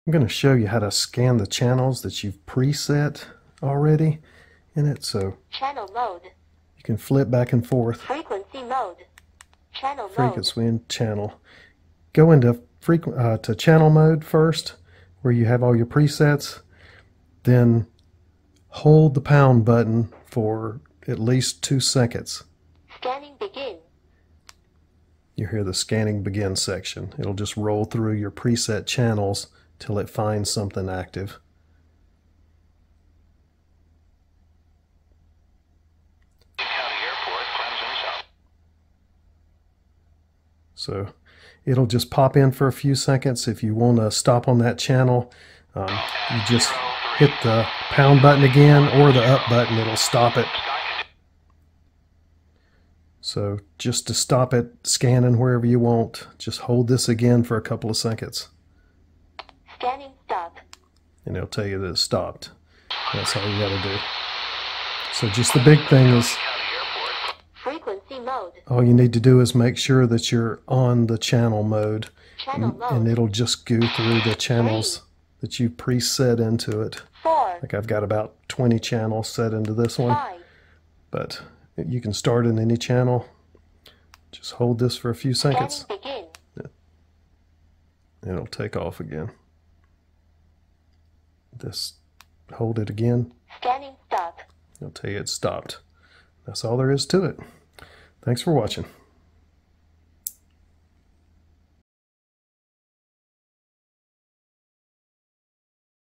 Baofeng 5RM Radio Scan Only Saved Channel Frequencies
Baofeng 5RM Radio Scan Only Saved Channel Frequencies.  This will show you how to scan only the channel frequencies that you have programed into your HAM radio.  This scanning method will work for many of the other newer Baofeng radios.
You can find the Baofeng 5RM and accessories